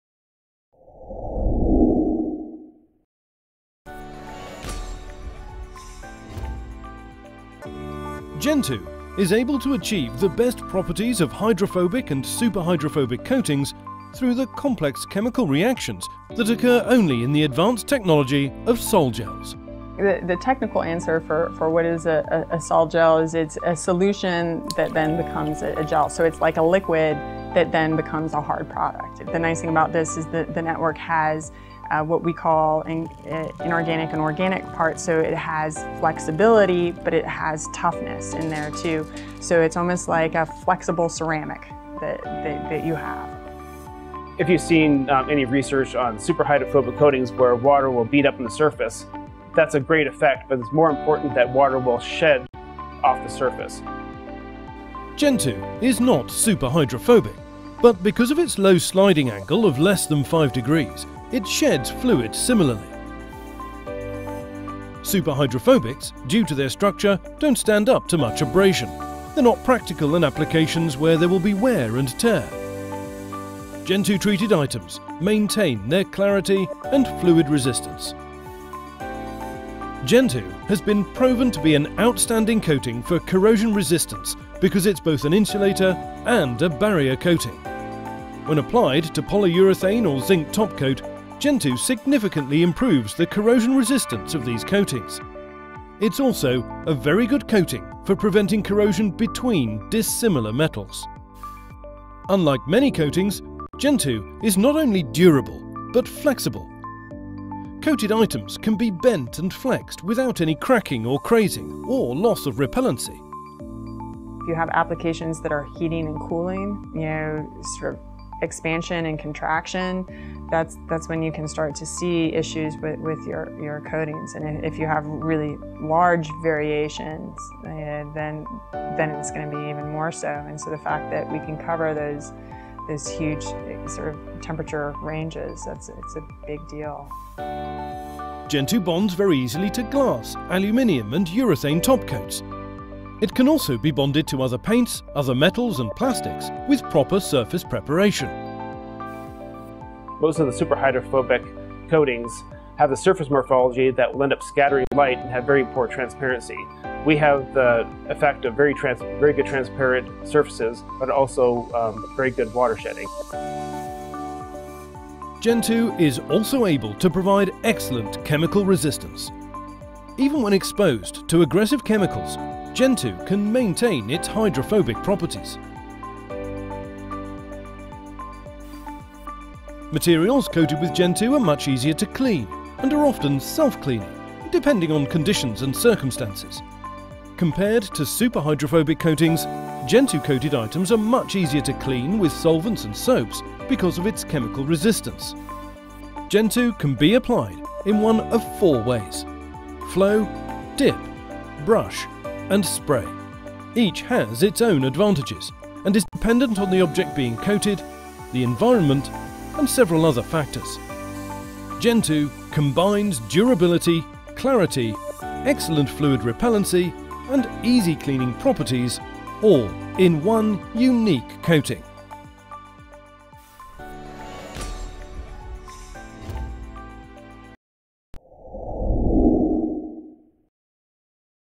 GENTOOCOATINGS - The Revolution In Coating - Maisam Trading LLC
Gentoo is the next generation of corrosion resistant and easy cleaning coatings. With its combination of high performing abrasion resistance and very low sliding angle, Gentoo excels where other coatings have fallen short.

Gentoo is able to achieve the best properties of hydrophobic and superhydrophobic coatings through the complex chemical reactions that occur ONLY in the advanced technology of sol-gels.

Gentoo is not superhydrophobic but because of its low sliding angle of less than 5 degrees, it sheds fluid similarly.

Superhydrophobics, due to their structure, do not stand up to much abrasion. They're not practical in applications where there will be wear and tear. Gentoo treated items maintain their clarity and fluid resistance.

Gentoo has been proven to be an outstanding coating for corrosion resistance because it is both an insulator and a barrier coating. When applied to a polyurethane or zinc top coat, Gentoo significantly improves the corrosion resistance of these coatings. It is also a very good coating for preventing corrosion between dissimilar metals.

Unlike many coatings, Gentoo is not only durable but flexible.  Coated items can be bent and flexed without any cracking or crazing or loss of repellency.

Gentoo bonds very easily to glass, aluminum and urethane top coats. It can also be bonded to other paints, other metals and plastics with proper surface preparation.

Gentoo is also able to provide excellent chemical resistance.

Even when exposed to aggressive chemicals, Gentoo can maintain its hydrophobic properties.

Materials coated with Gentoo are much easier to clean and are often "self-cleaning" depending on conditions and circumstances. Compared to superhydrophobic coatings, Gentoo coated items are much easier to clean with solvents and soaps because of its chemical resistance.

Gentoo can be applied in one of four ways, flow, dip, brush and spray. Each has its own advantages and is dependent on the object being coated, the environment and several other factors.